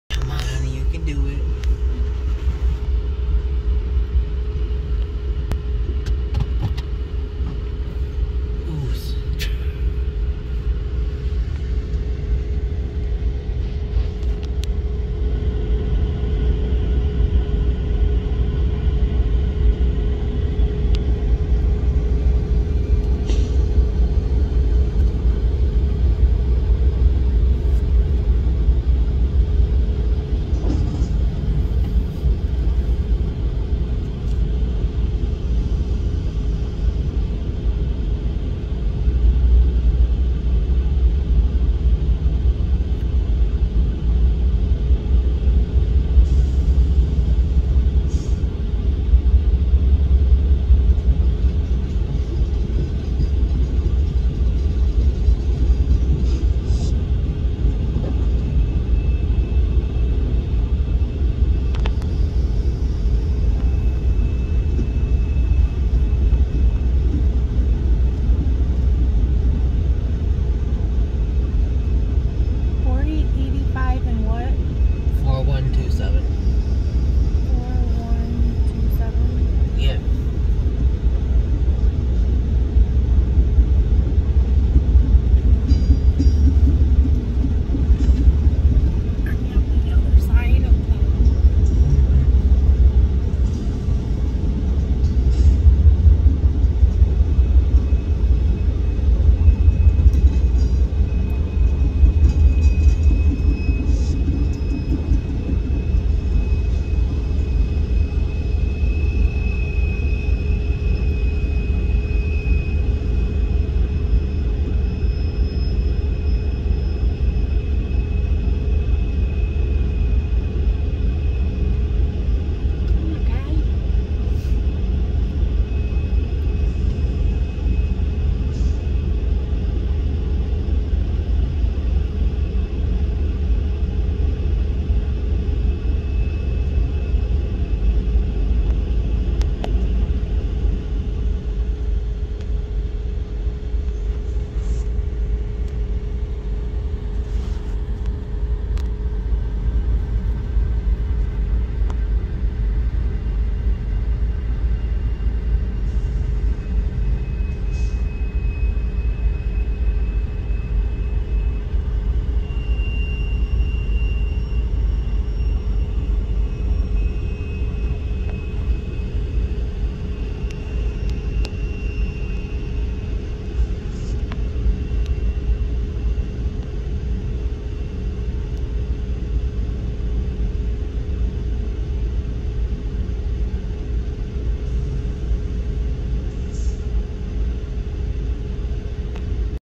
Came back to the rail park to find a train blocking the crossing in Bellevue on 8/28/2021
NS 4127 and 4085